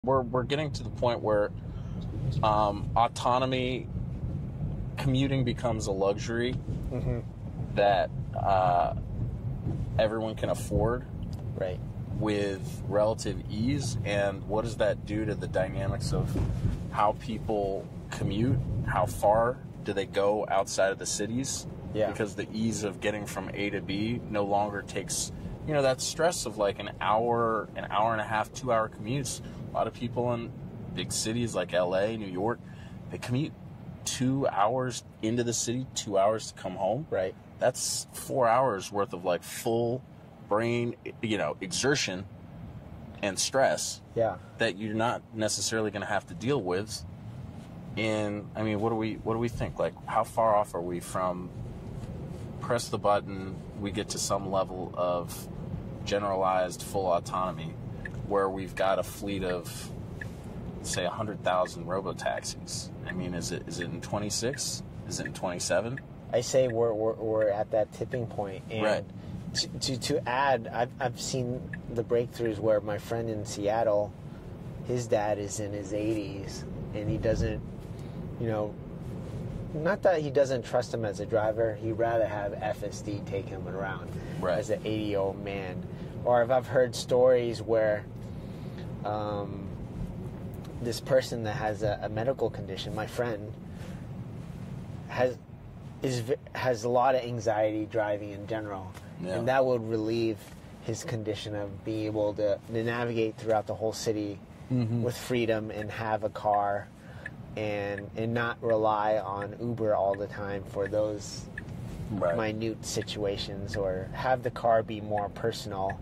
"Commuting becomes time you get back, not time you lose.” DTP FSD S02EP03 Trailer
Autonomy is about to flip commuting from a daily stress tax into a cheap luxury anyone can access.

In this clip, we talk about what happens when getting from A to B stops demanding “full brain exertion” for hours a day. In cities like LA and New York, people regularly burn 2 hours in and 2 hours back. That is four hours of stress, attention, and fatigue. Now imagine pressing a button and arriving calm.

We also dig into what this does to the map of where people live, how far they’ll travel, and how quickly robo-taxi fleets could scale (is it 2026? 2027?). Plus, real human proof: older riders trusting FSD more than a tired driver, and people with driving anxiety regaining freedom and mobility without living on Uber.

If autonomy commuting becomes a luxury everyone can afford, cities, suburbs, and daily life do not stay the same.

Car: 2025 ‪Tesla‬ Model 3 with hardware 4 (ai4)
FSD (Supervised) v14.1
Camera: ‪DJI Osmo Action 5 Pro
Executive Producer: C.Y. Lee
📍 Los Angeles, CA, USA

David T Phung
Presented by NLT143